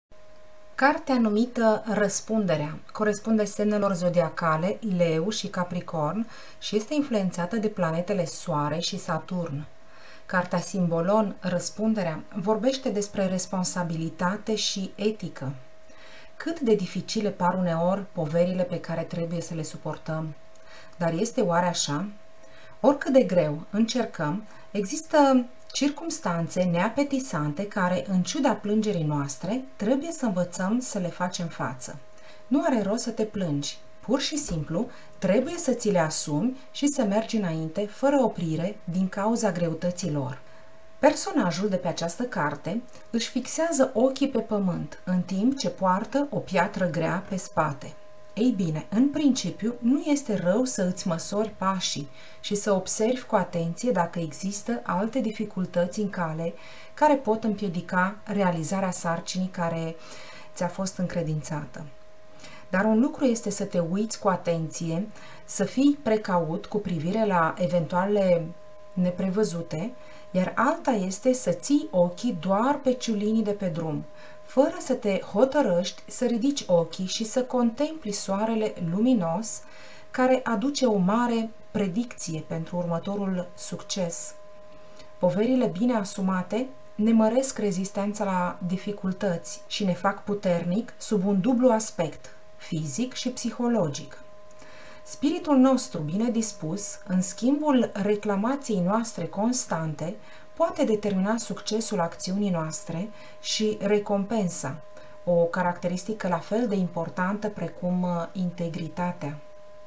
SYMBOLON Răspunderea: Leu, Capricorn, Soare, Saturn.
Cartea Symbolon Răspunderea vorbește despre: responsabilitate, etică.